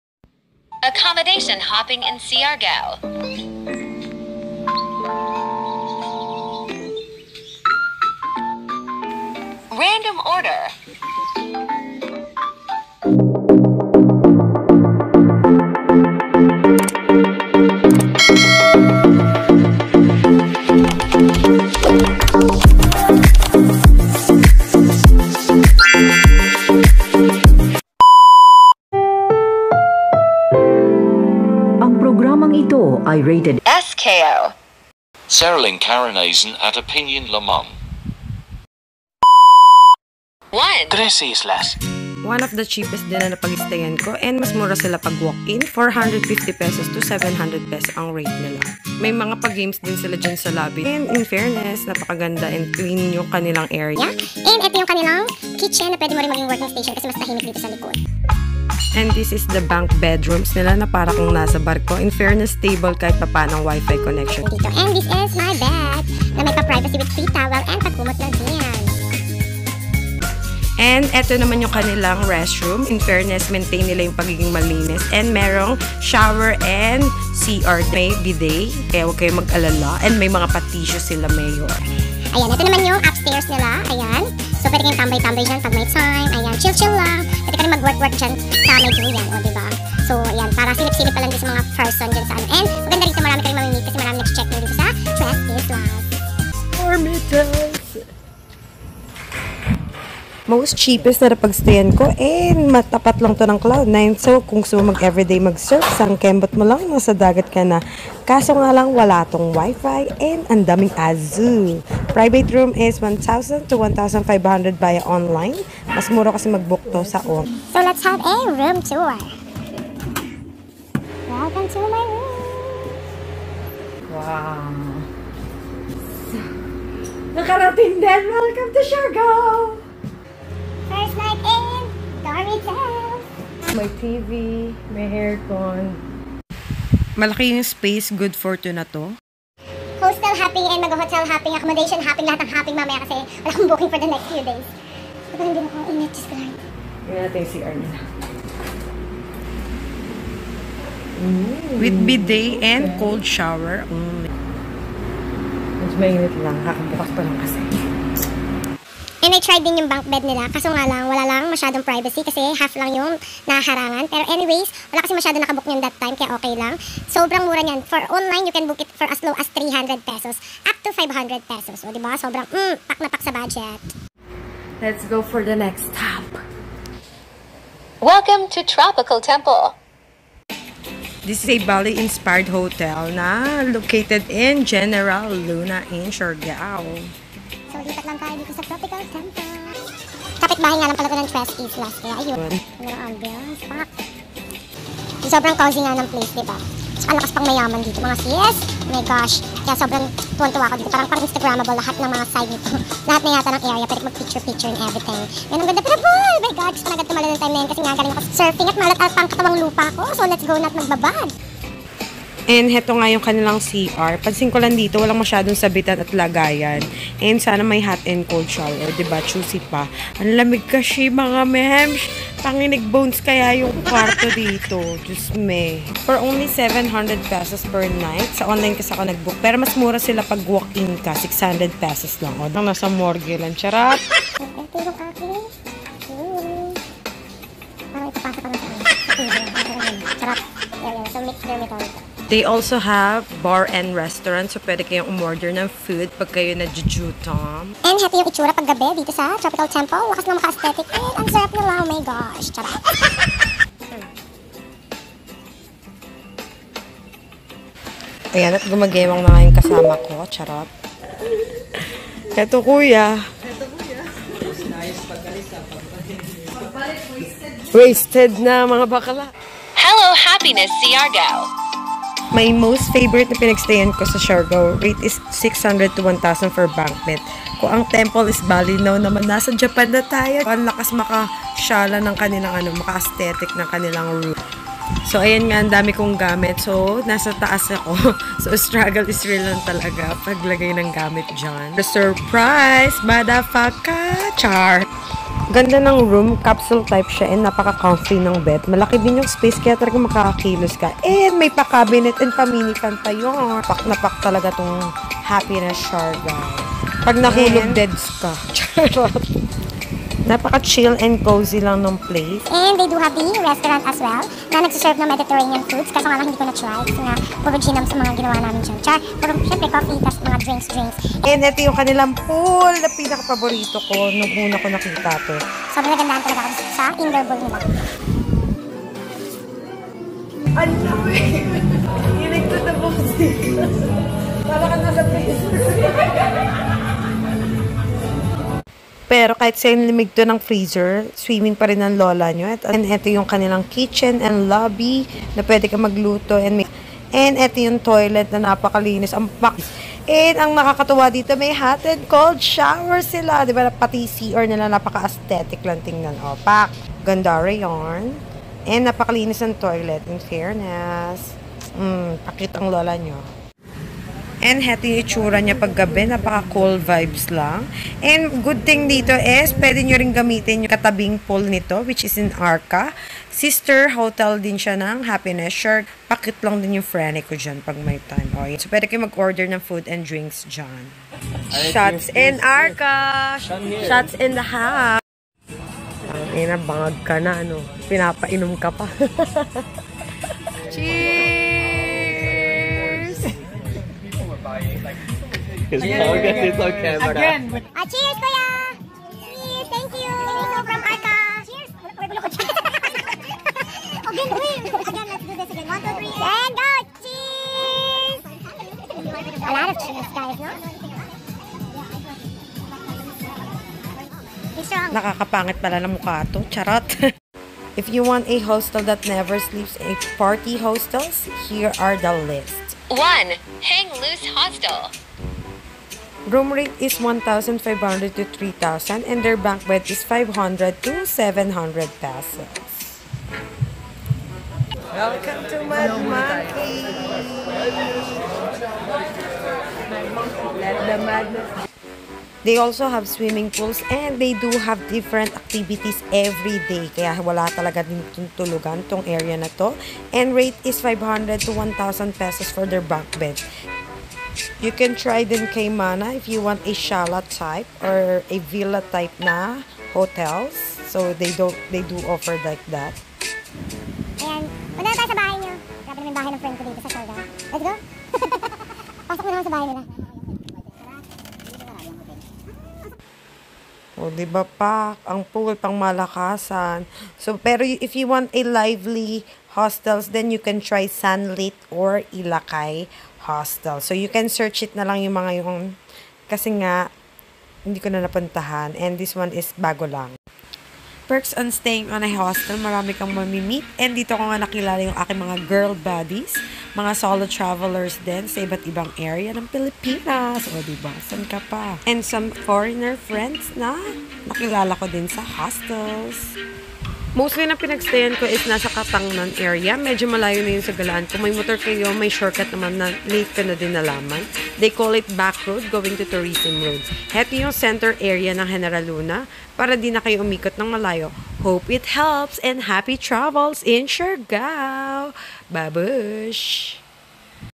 Where to Stay in Siargao Philippines l on a Budget l Tips and Expenses l 2023
Those reviews are based on my experience last time Ive went there March 2023
Hope my vlogs help you on choosing your accommodation in Siargao♥️

Cheapest Hostel:
Dormitels ph
Tres Islas

Bali type Hotel: Tropical Temple
Japan Type: Happiness Siargao

Party Hostels:
Mad Monkey
Hang Loose
Sunlit
Ilakai

Villa Type: Kaimana

I put the rates on the video
Most of it is in Catangnan area in General Luna

Happy Travels
Appreciate your feedback too♥️